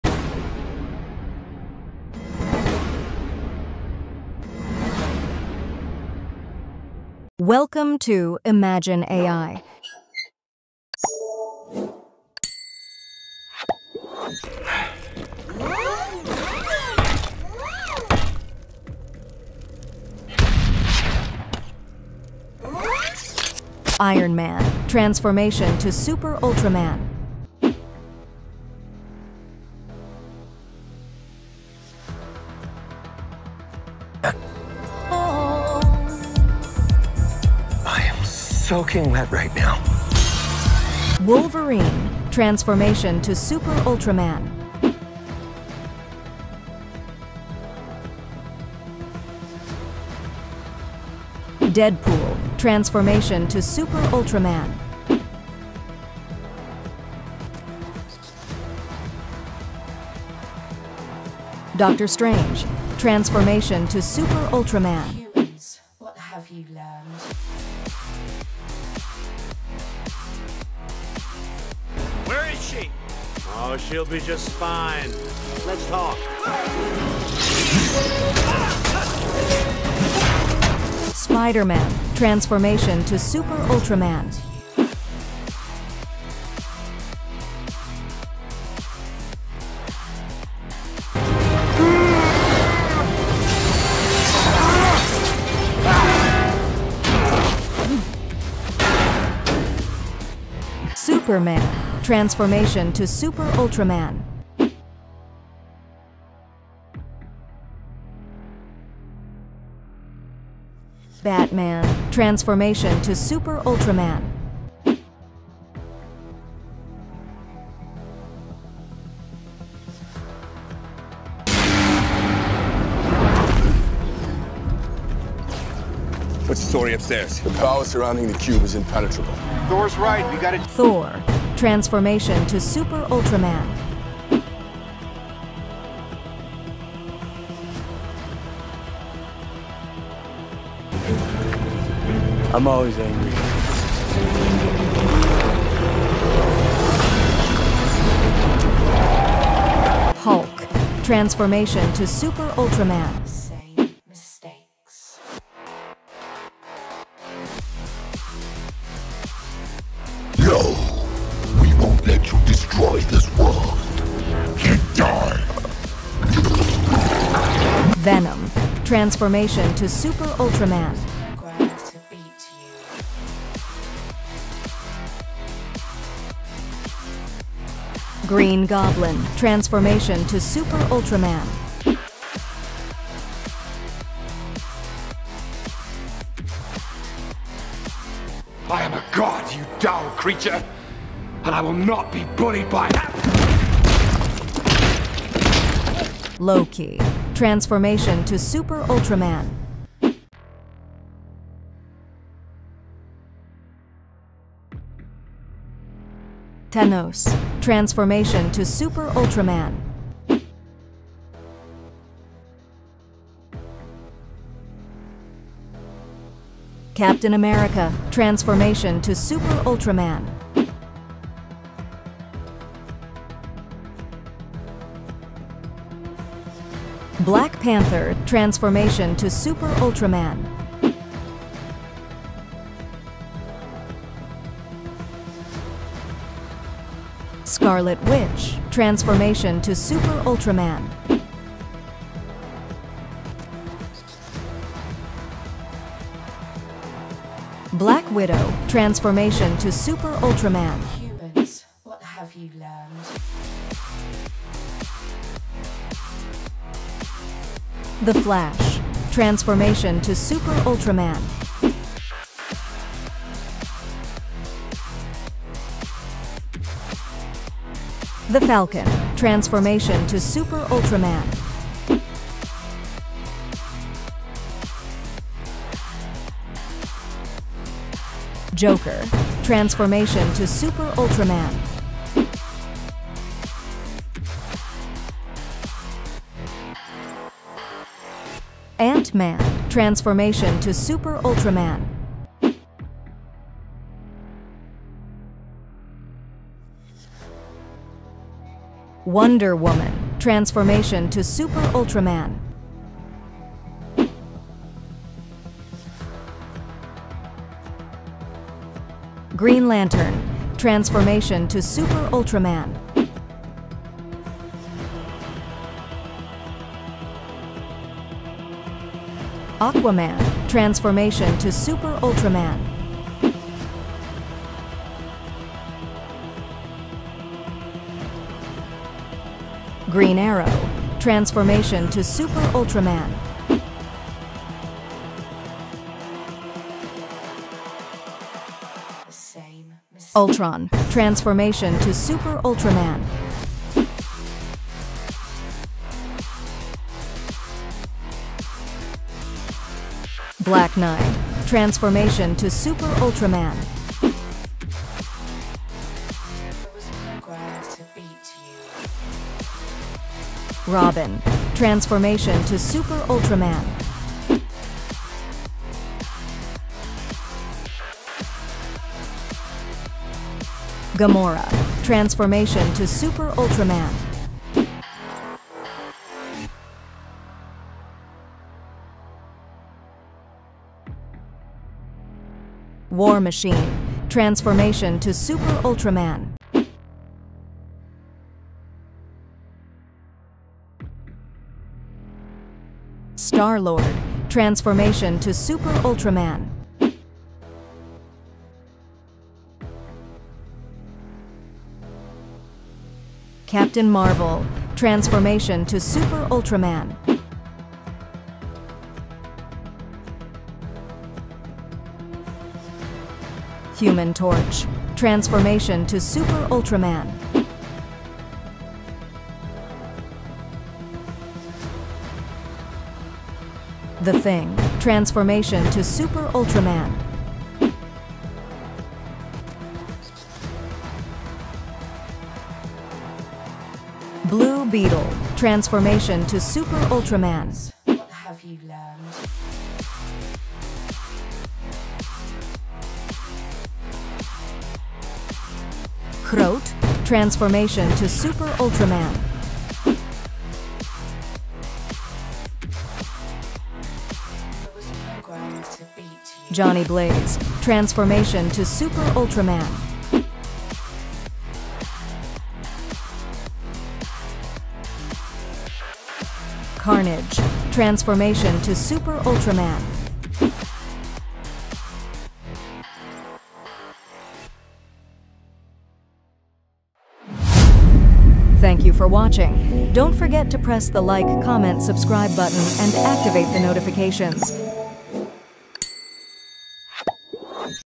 AVENGERS but ULTRAMAN VENGERS 🔥 All Characters (marvel & DC) 2024 😎
So the content of my video is about imagining, right?
The one on the left is the original version, the one on the right is the AI ​​version that I made to imagine the original character, thank you for watching my video, I hope you have fun.

Thank you to Marvel & DC Studio, I really respect the work of Marve & DC Studio,
ᵗᑋᵃᐢᵏ ᵞᵒᵘ*🎀🫶🏻💌💓